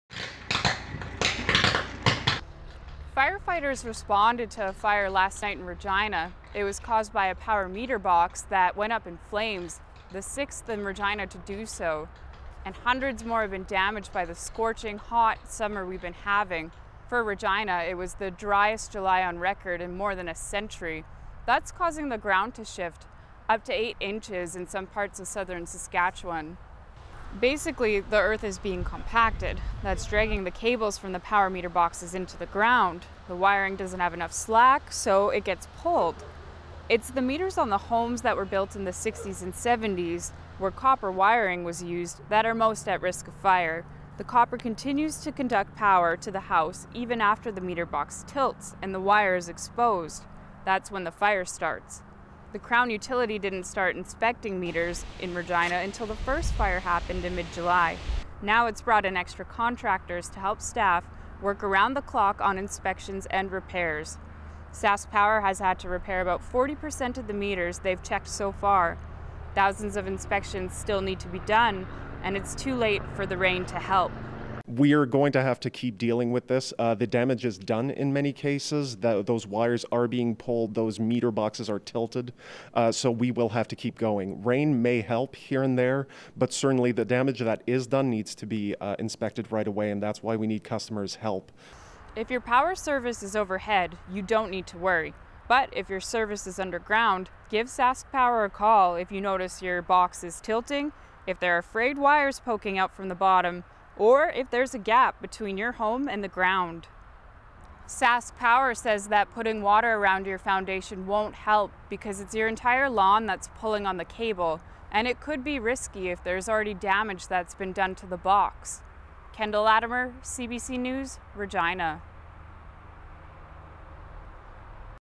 Of the 2,000 meters SaskPower has inspected, 40% required repairs
SaskPower has now expanded meter inspections to areas with soil similar to Regina's, like Moose Jaw, and there may be the possibility of work done in Shaunavon and Kindersley as well.

Credit
Journalist: Kendall Latimer
Videographer: Mike Zartler, Adam Hunter